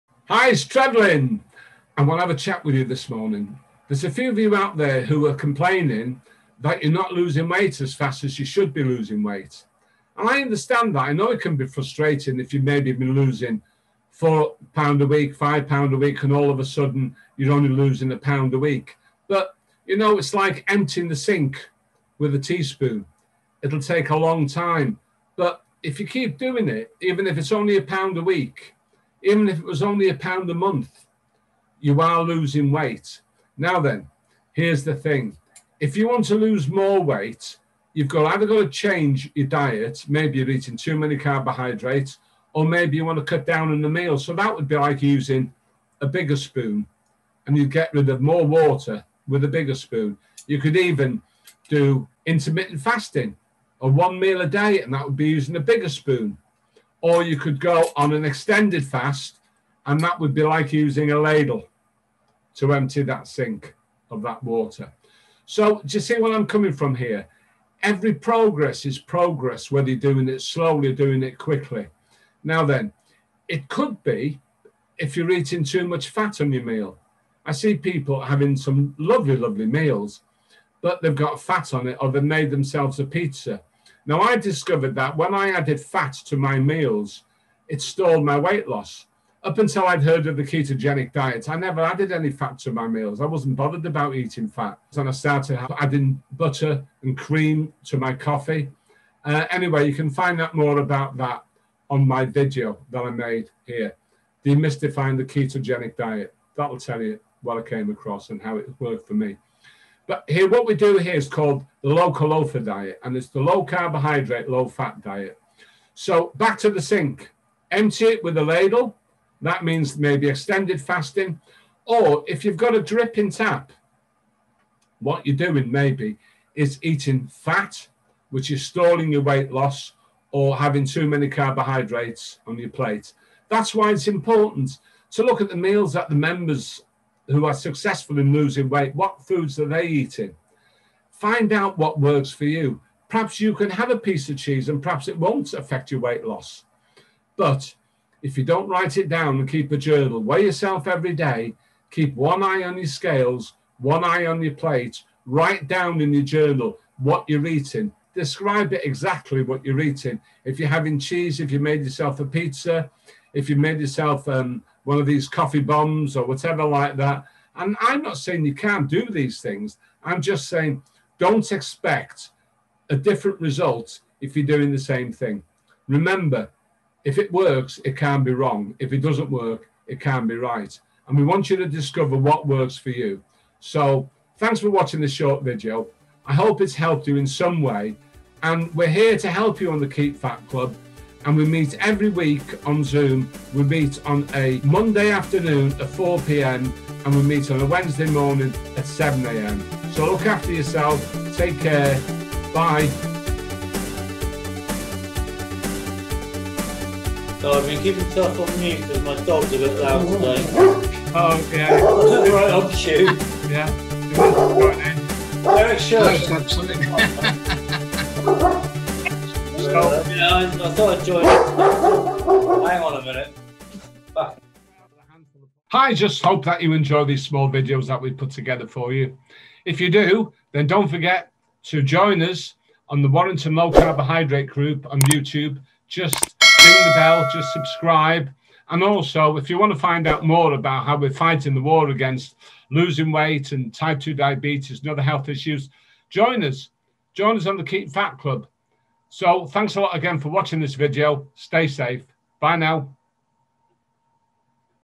HAS YOUR WEIGHT LOSS STALLED ???
There is so much advice and information available today on how to lose weight. There's only one way to lose weight and that is to moderate what we are eating. Find people who have experience based evidence of what worked for them. So many people have joined us on the KEEP FAT CLUB looking for answers.
Sometimes people's weight loss stalls and they need help. encouragement and support.
I made this video to try to provide a simple illustration of what might be happening when our weight loss stalls.  It's not full of science and technical jargon. Just a simple way that I have tried to explain things.